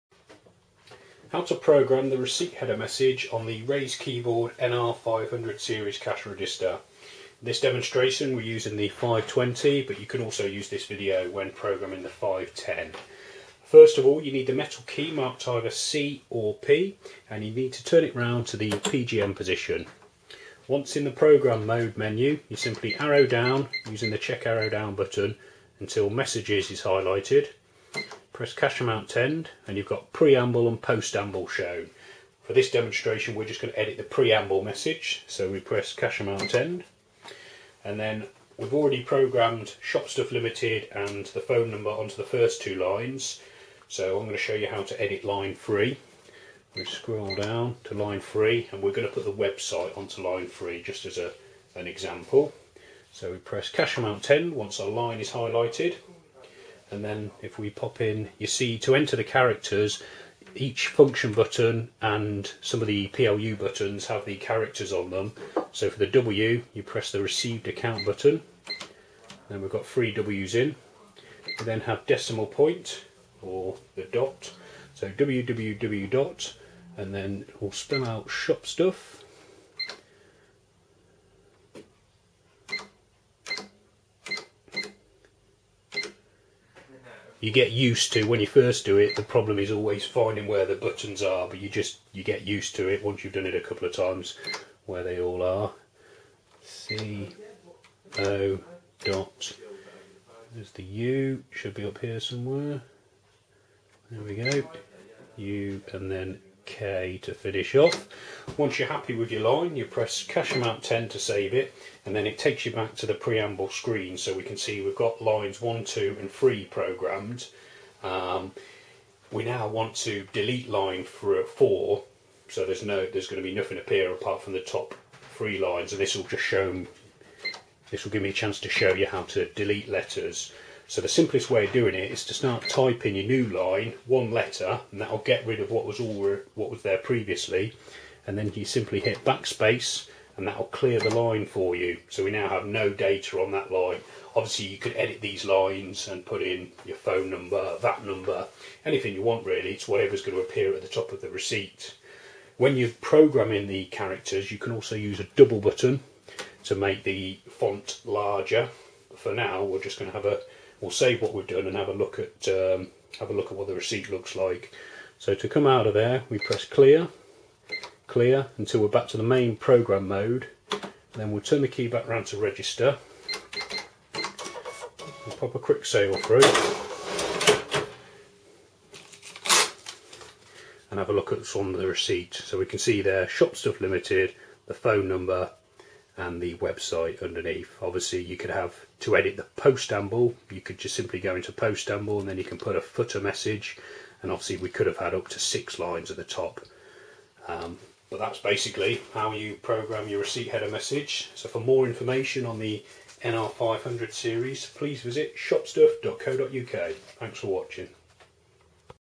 HOW TO PROGRAM THE RECEIPT HEADER MESSAGE ON THE NR-510 OR NR-520
How to program the receipt header message on the NR-510 or NR520 cash register.